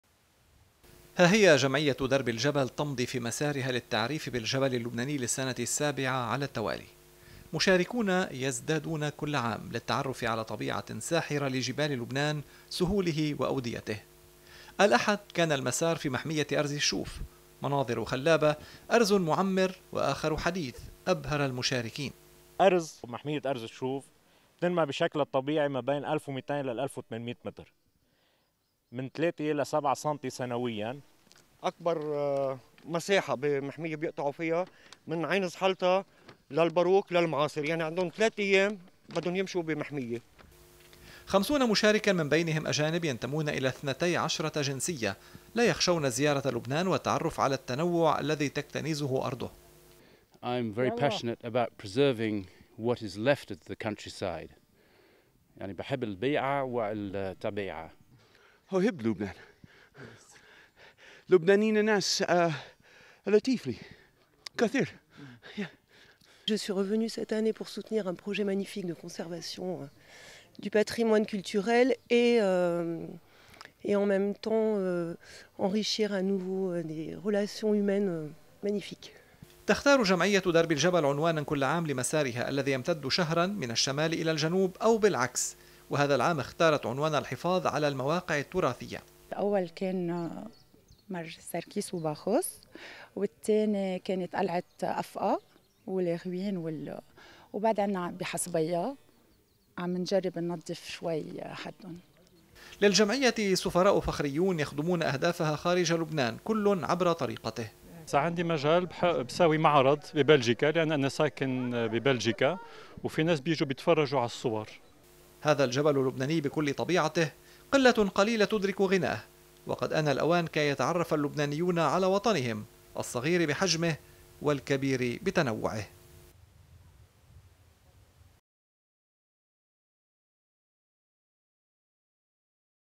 LBCI News - بعد 7 أعوام... درب الجبل مستمرة!
تمضي جمعية درب الجبل في مسارها للتعريف بالجبل اللبناني للسنة السابعة على التوالي، ومشاركون يزدادون كلّ عام للتعرّف على طبيعة ٍ ساحرة لجبال لبنان سهوله واوديته.